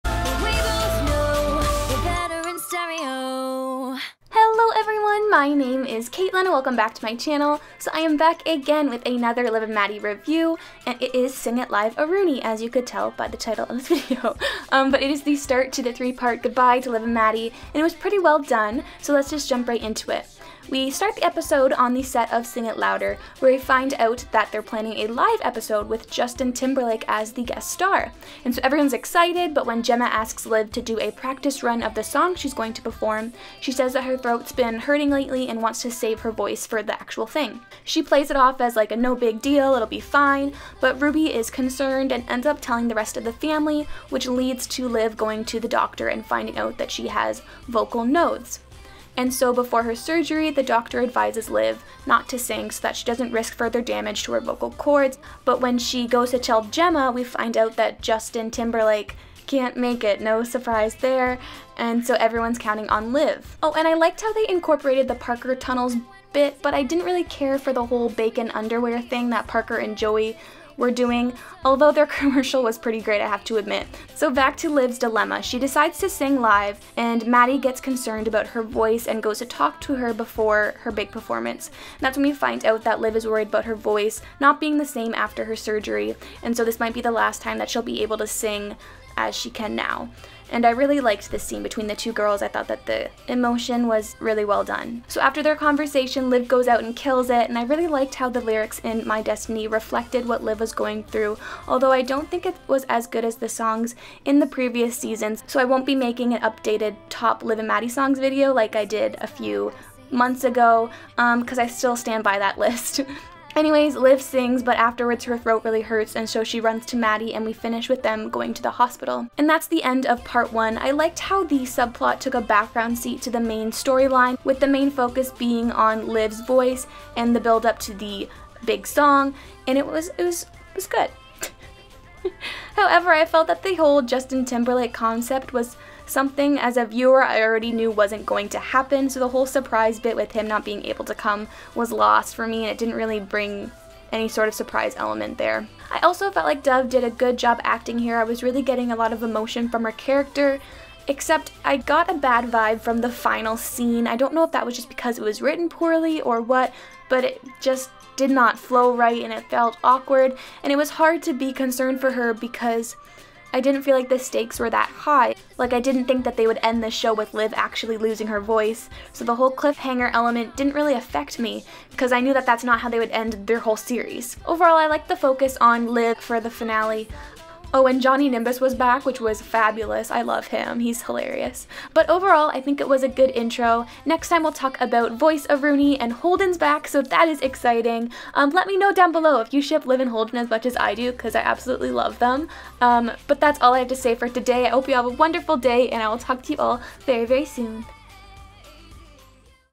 Sing it Live!!-A-Rooney!! Liv and Maddie: Cali Style Review!!
My thoughts on the first stop on the Liv and Maddie goodbye train, pretty bittersweet for me, what did you think of this episode?

let's be pen pals!! 🥰
Caitlin McKillop
Broadway PO PO
Toronto, ON, CA
M4P 0A8